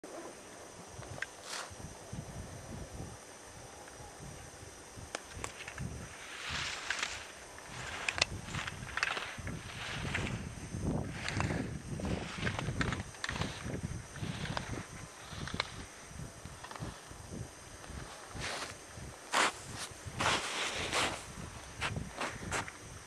Day 2 of Slalom training at Mt. Hood
Learning to block gates, keep upper body open to hill, keep outside ski weighted.